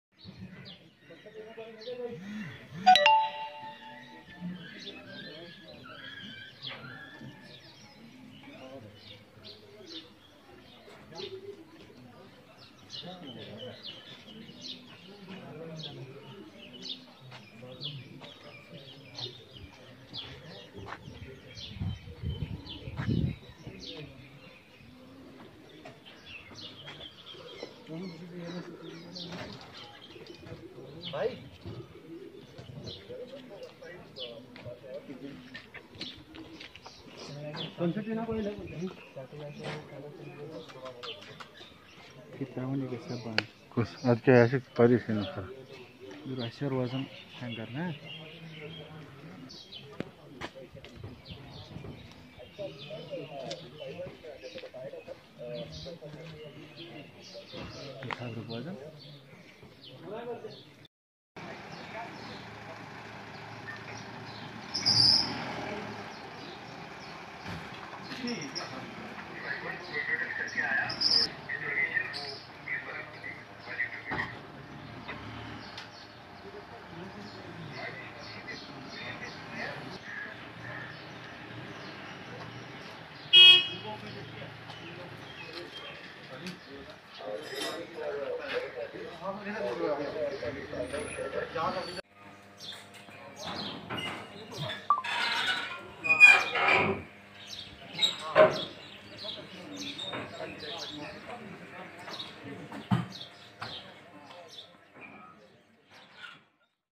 Khanmoh: Joint operation was launched early morning today based on J&k Police inputs, Area cordoned
Khanmoh: Joint operation was launched early morning today based on J&k Police inputs, Area cordoned and contact established. Firefight ensured.. Joint operation in progress | JK News Today